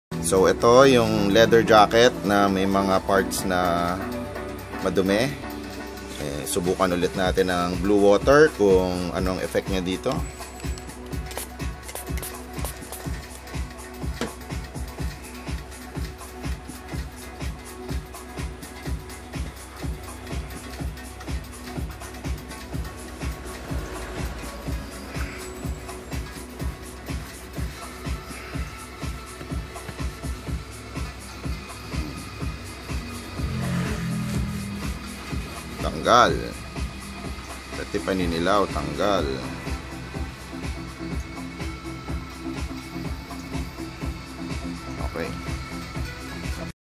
blue water on leather jacket
the blue water cleaning solution also works wonders for leathers